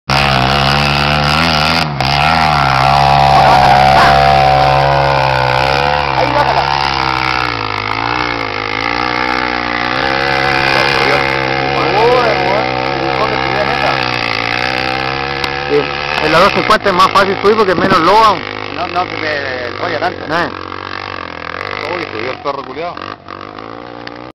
Enduro Antofagasta - Koke en moto ajena 2
ahora haciendo K-gar la XR 250 de Alberto